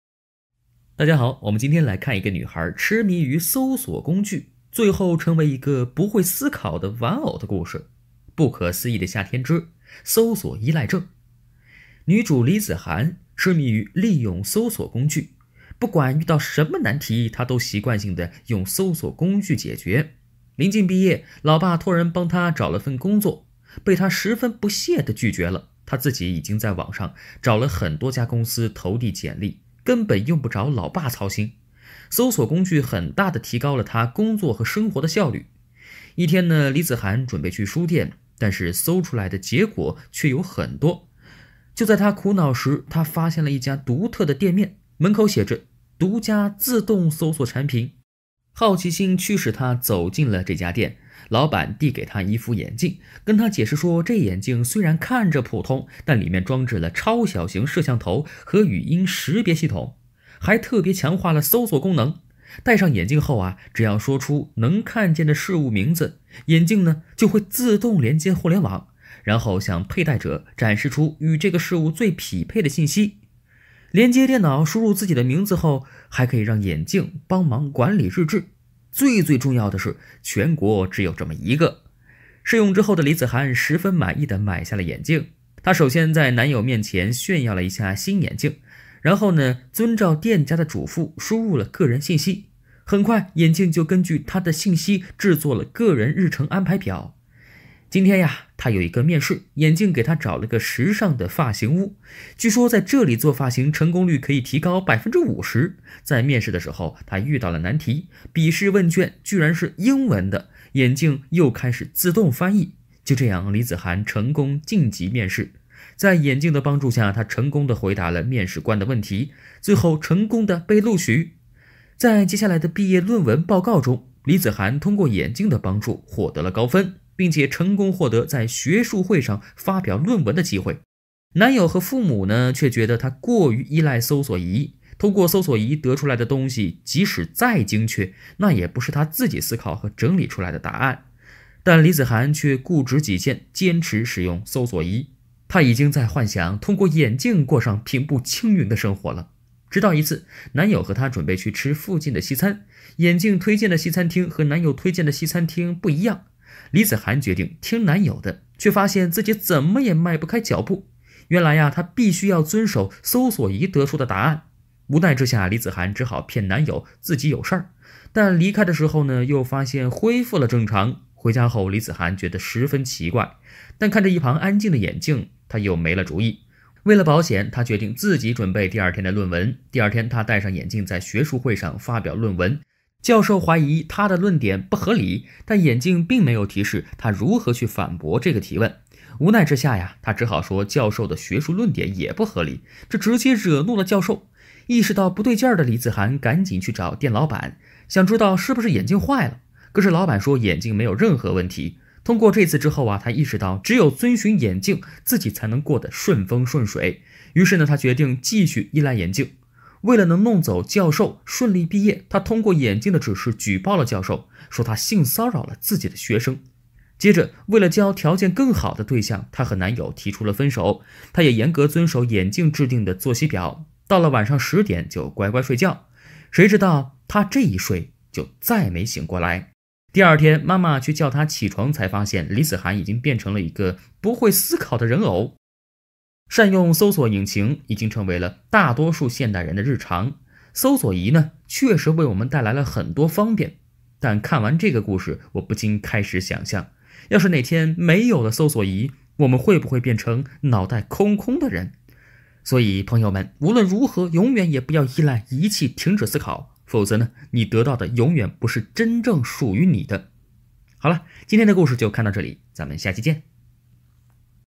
女孩痴迷使用搜索工具，从来不自己思考，最后变成不会思考的人偶
电影、影视、奇幻、惊悚、剧情